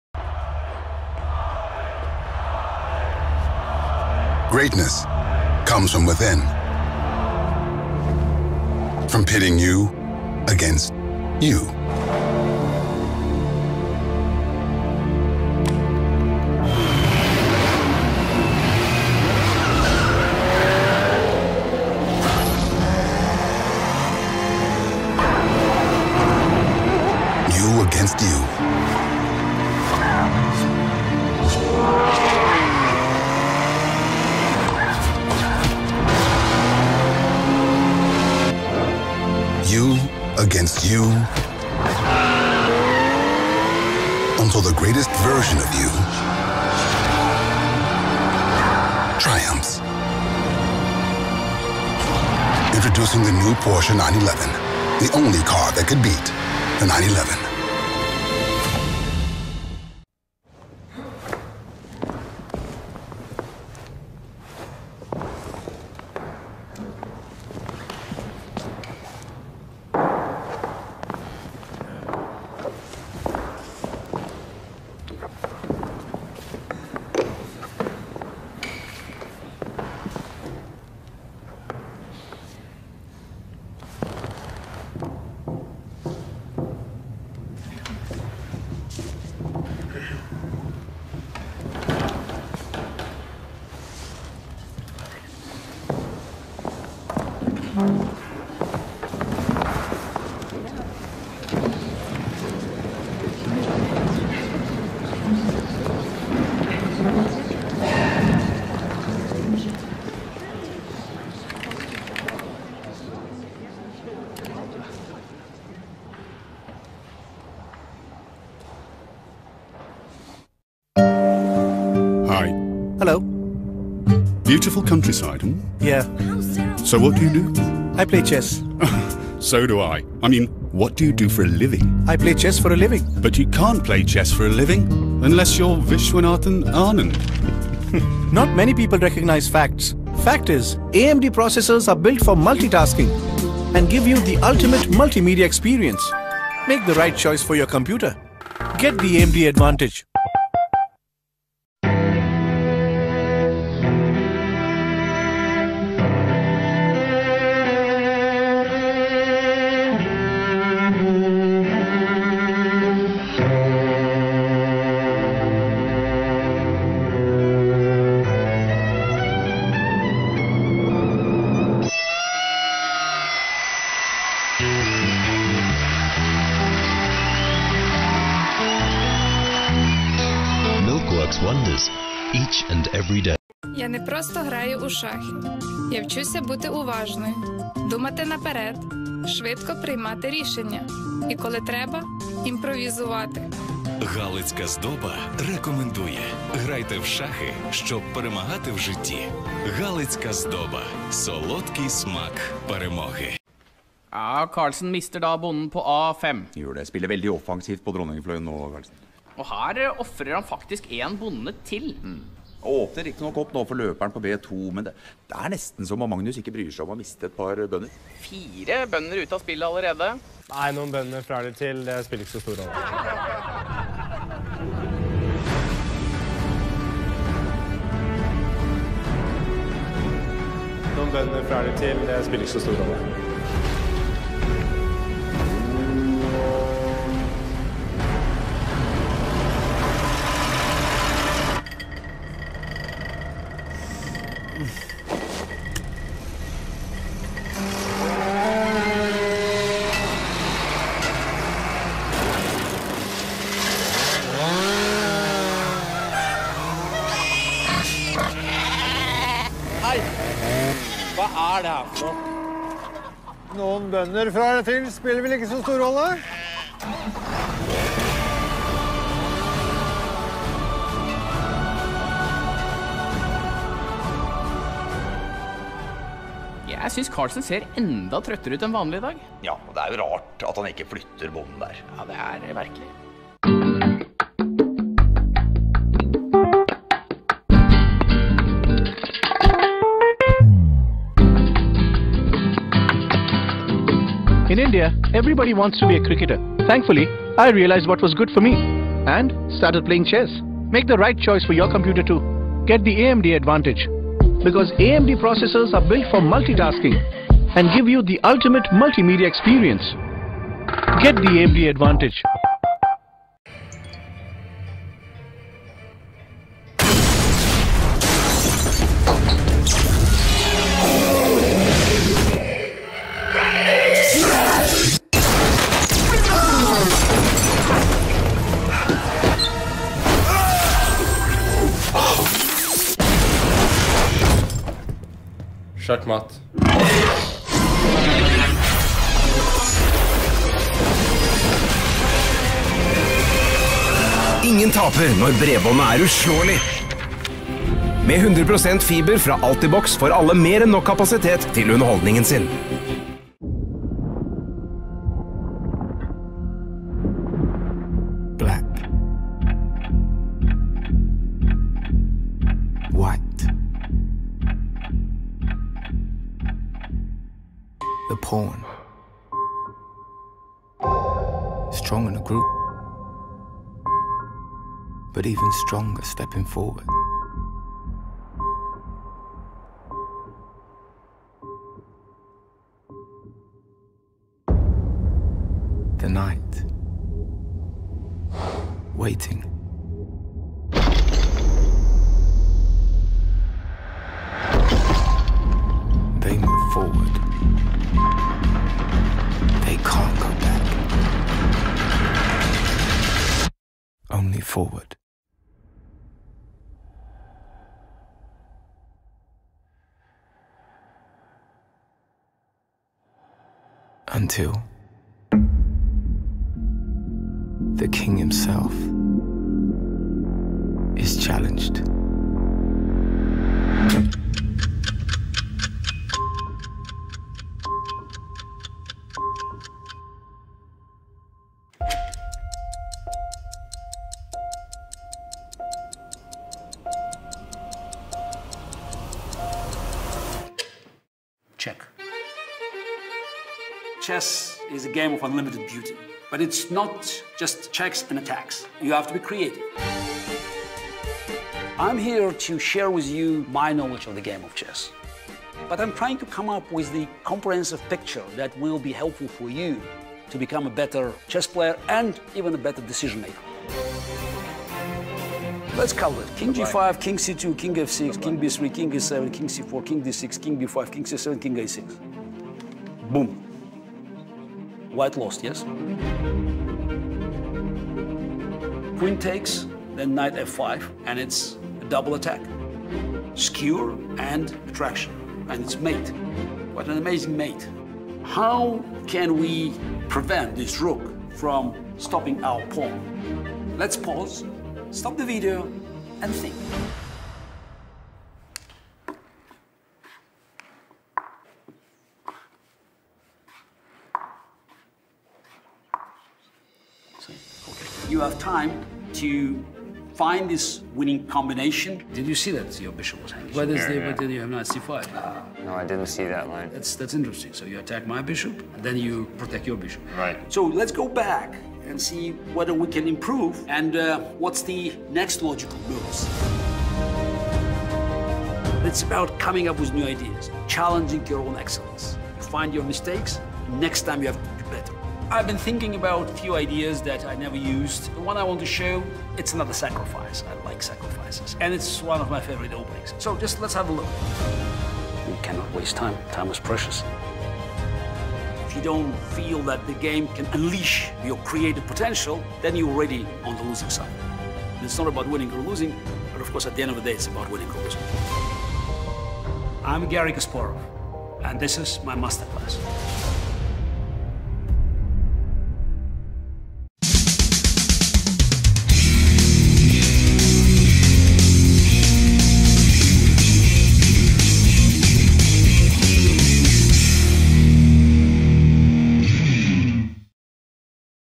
Chess Commercials Compilation | Part 1 - Famous Players Edition
Magnus Carlsen, Garry Kasparov, Viswanathan Anand, Viktor Kortchnoi and Mariya Muzychuk star in these commercials!

This is a compilation of the best chess commercials of all times: Famous Players Edition.

0:00 Porsche (Magnus Carlsen & Maria Sharapova)
1:00 AltaVista (Garry Kasparov)
2:00 AMD (Viswanathan Anand)
2:30 Milk (Viktor Korchnoi)
3:02 Coffe (Mariya Muzychuk)
3:23 Felleskjopet (Magnus Carlsen)
4:43 AMD (Viswanathan Anand)
5:12 Altibox (Magnus Carlsen)
5:53 G-Star.com (Magnus Carlsen)
7:11 Masterclass (Garry Kasparov)

This is Part 1 of the Best Chess Commercials compilation.